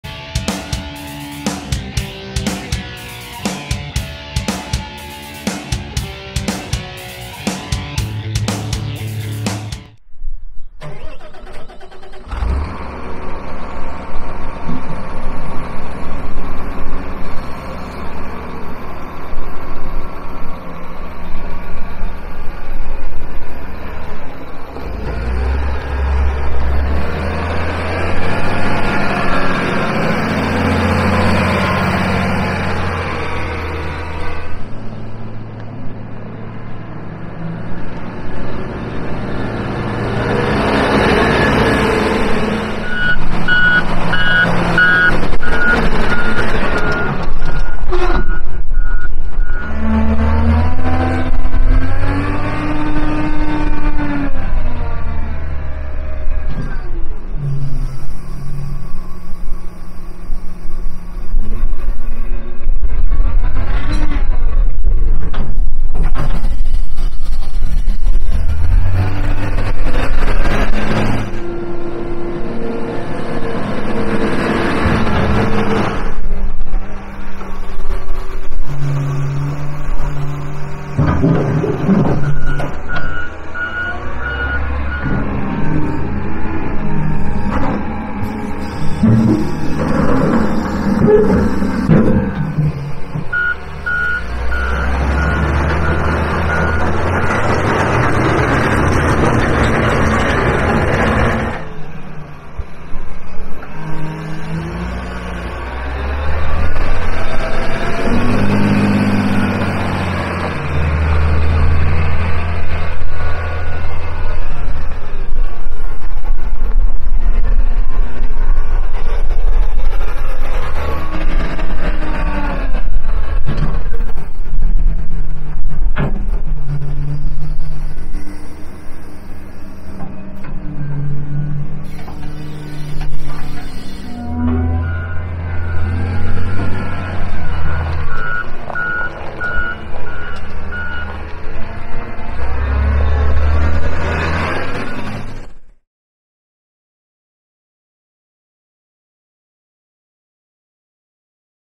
2012 John Deere 644K
Cab w/ Heat & AC, JRB 418 QC, Aux Hydr, Joystick Control w/ F/R, 23.5x25 Tires, 232hp.

Fleet trade in. Clean and tight, excellent running loader. Has all of the options and is very well maintained.